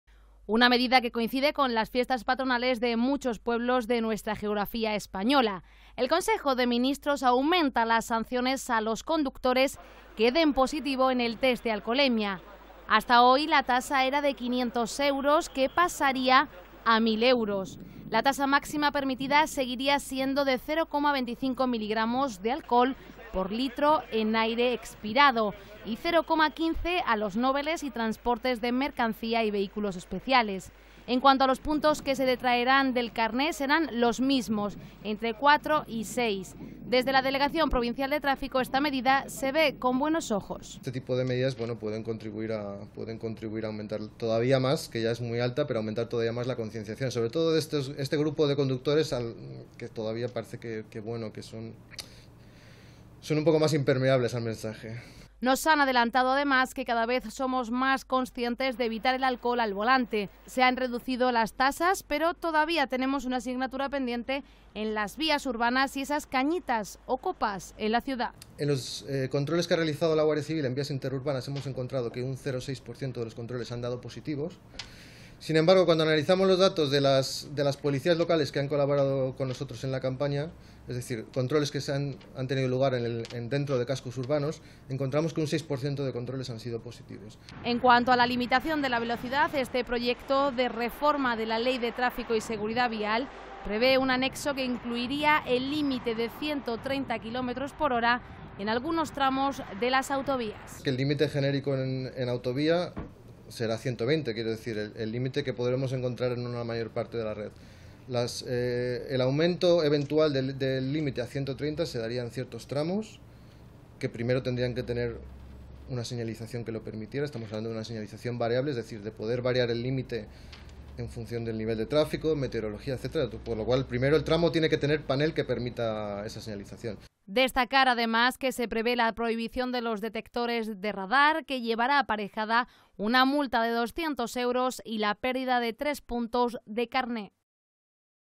Aumentan las sanciones a los conductores que den positivo en el test de alcoholemia
Una medida que coincide con las fiestas patronales de muchos pueblos. El Consejo de Ministros aumenta las sanciones a los conductores que den positivo en el test de alcoholemia; hasta hoy la tasa era de 500 euros que pasarán a 1.000. La tasa maxima permitida seguirá siendo de 0.25 miligramos de alcohol por litro en aire expirado, y 0.15 a los noveles y trasporte de mercancias y vehículos especiales. En cuanto a los puntos que se detraerán del carné será los mismos entre 4 y 6. Desde la Delegación Provincial de Tráfico esta medida se ve con buenos ojos.
Nos han adelantado además que cada vez somos más conscientes de evitar el alcohol al volante se han reducido las tasas pero todavía tenemos una asignatura pendiente en las vias urbanas y esas cañitas o copas en la ciudad.
En cuanto a la limitación de la velocidad este proyecto de Reforma de la Ley de Tráfico y Seguridad Vial prevé un anexo que incluirá el límite de 130km/h en algunos tramos de las autovias.
Destacar además que se prevé la prohibición de los detectores de radar, que llevará aparejada una multa de 200 euros y la pérdida de tres puntos de carné.